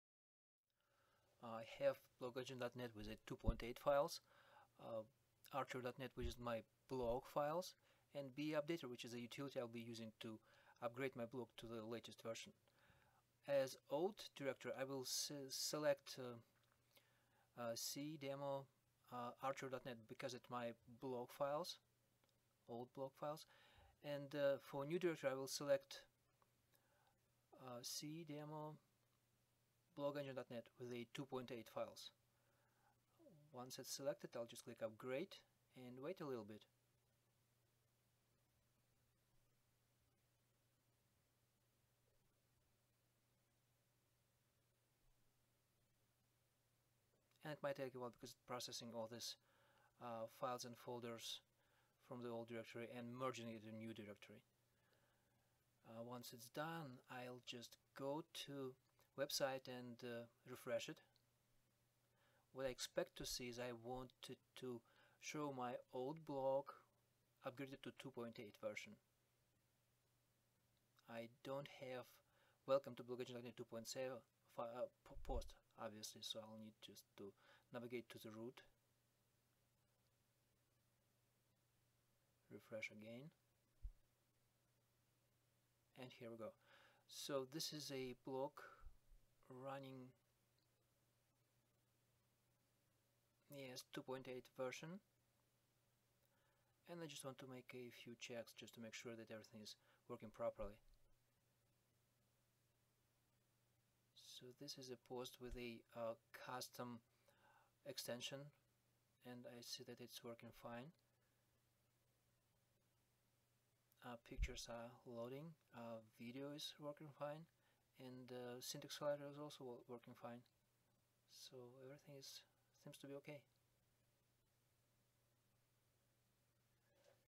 Upgrade from BlogEngine.NET 2.7 to BlogEngine.NET 2.8
Using upgrade utility automatically upgrade blog running BlogEngine.NET 27 to BlogEngine.NET 2.8